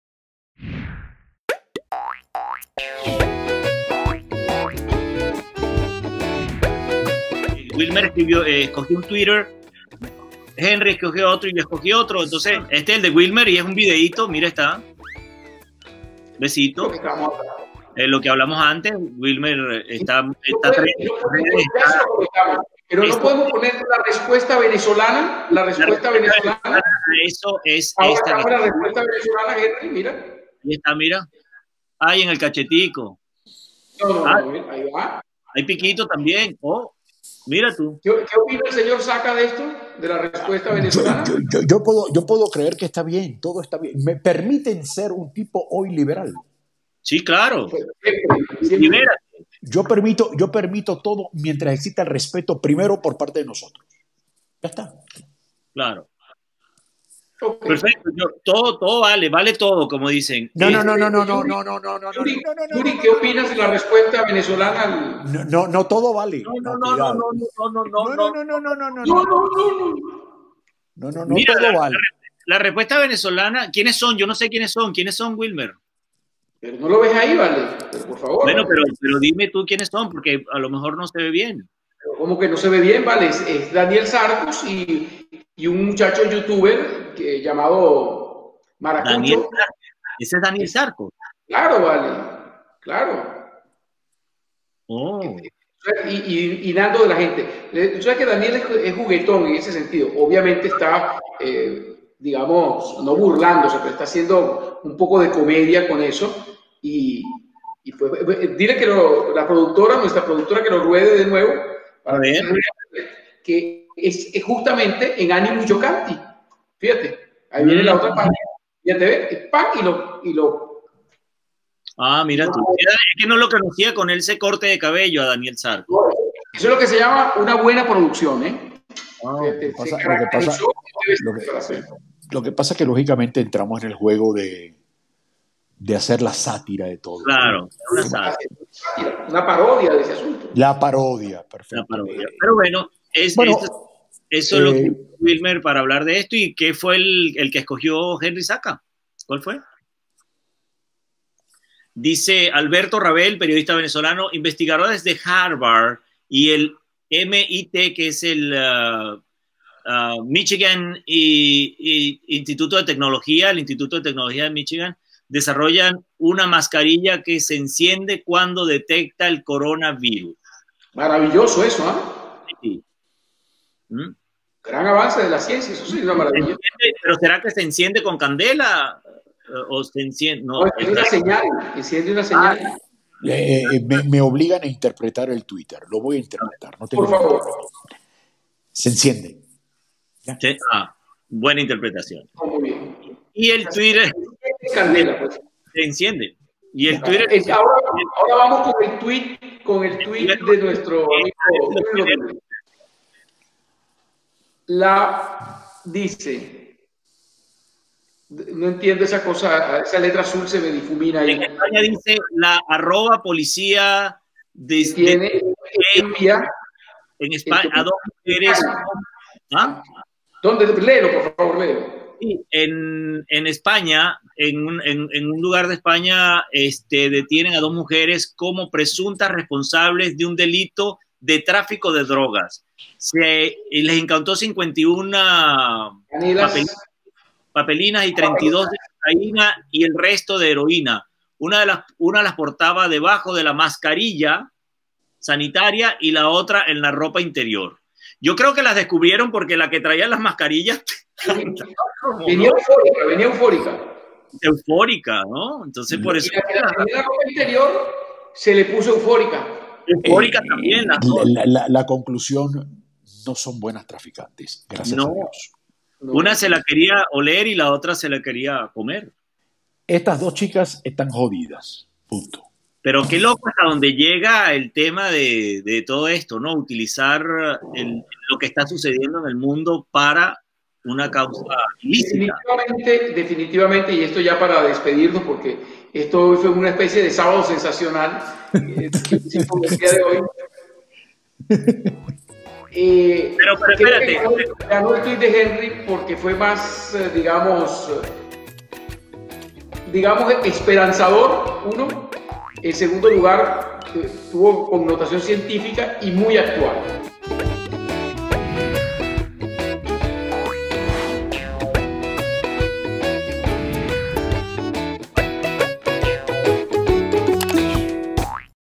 In de House Episodio 04 - Segmento Twitter
In de House con Henry Zakka, Wilmer Ramirez & Yuri Rodriguez!

Tres actores. Tres amigos. Tres cabezas... brillantes.

Grabado en vivo por Facebook Live Mayo 15, 2020 desde Los Angeles, Miami y ciudad de Mexico.

FB.com/indehouselive